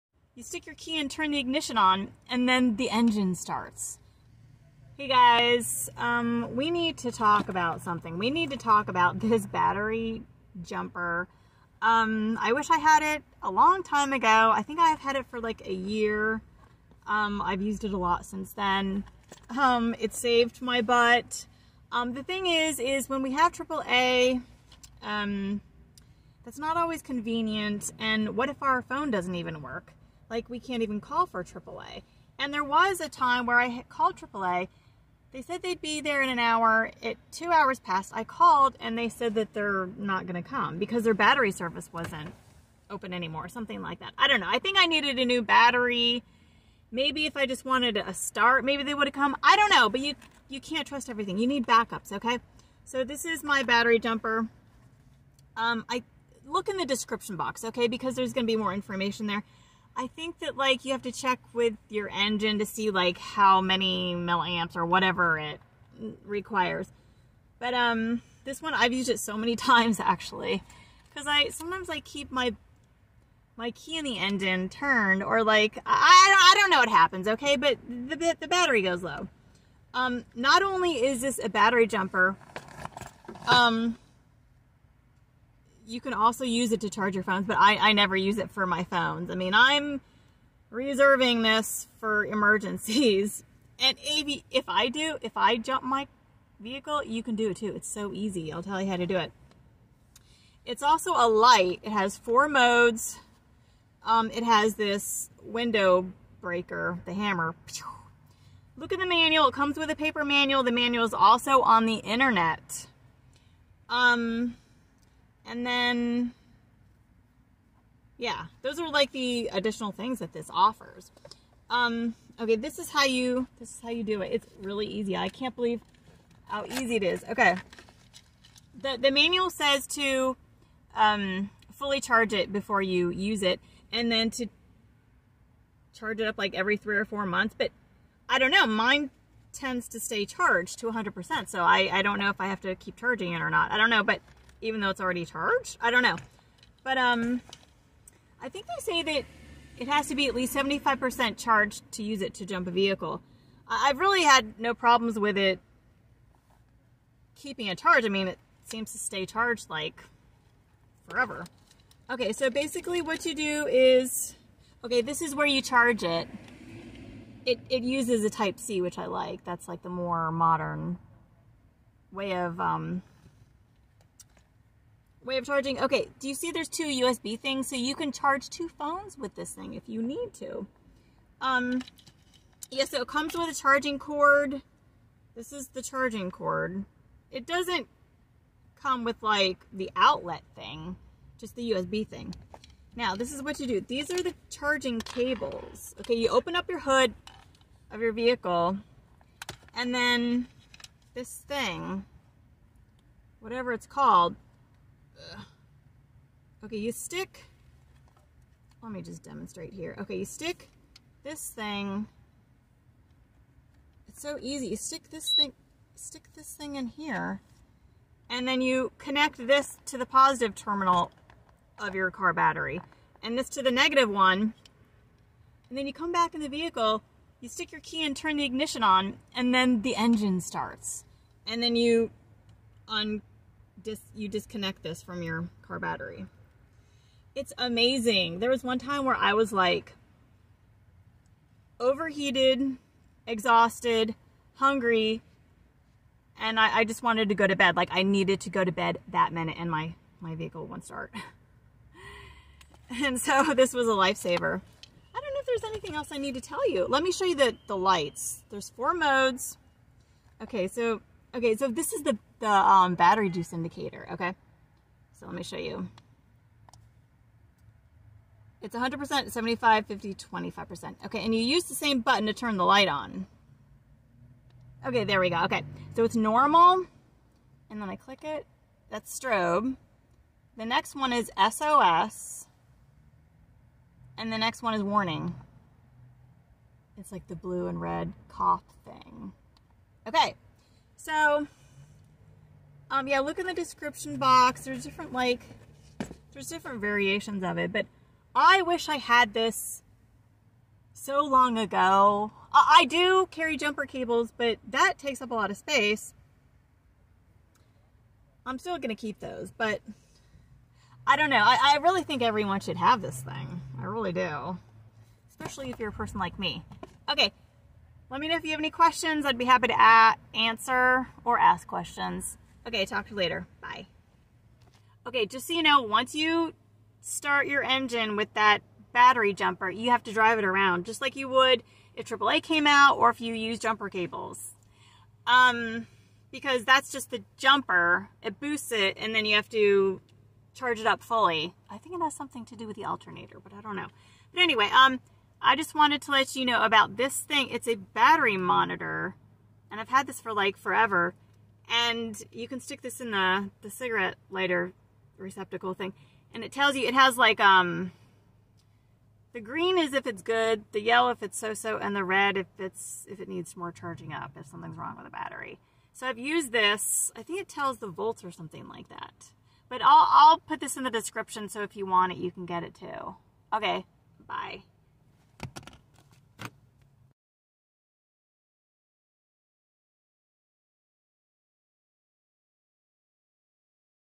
ESSENTIAL vehicle battery jumper review Utrai 16,000 mAh | I won't go without it.
I will not go anywhere without my Utrai 16,000 mAh car battery jumper. It has saved me a bunch of times! I totally endorse this. If your car won't start, and your battery is dead, this might be able to save you! But don't forget to also get your battery checked out by AutoZone to see if you need another battery.

You can email them to make sure you get the right amperage. It has to do with the power in your engine and possibly the CCA (cold cramping amps) listed on your car battery 🔋.

(20,000 mAh) No window breaker or red/blue 🚨 mode on lights, but it has a voltage meter right on it.